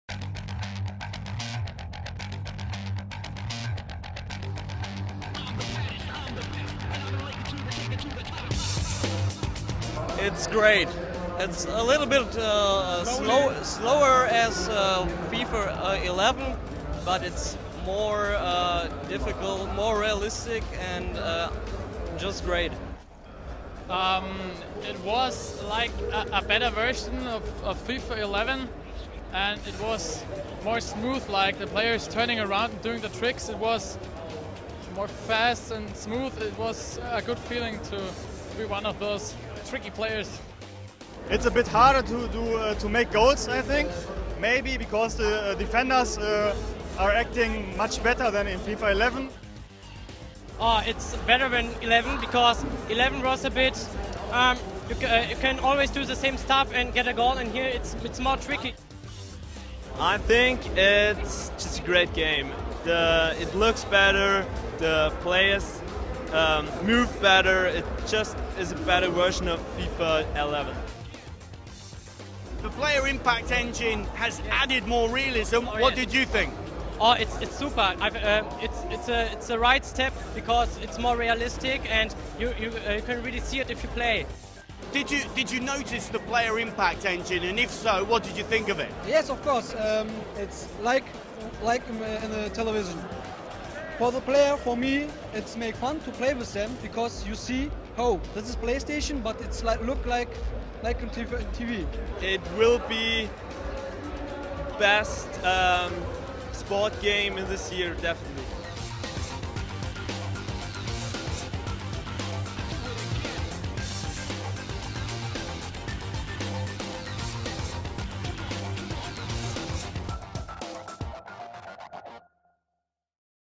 FIFA 12 - Le prime impressioni dal Gamescom 2011 - Italiano
Gamescom 2011 - Le prime impressioni direttamente dai giocatori che hanno toccato con mano FIFA 12!